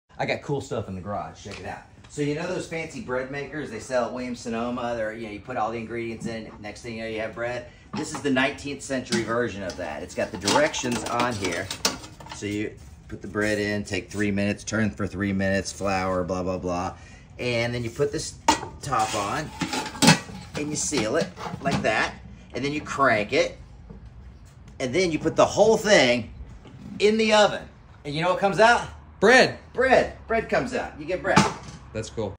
19th century bread maker
Paul brown shows off his bread making kit from the 19th century. Just add bread.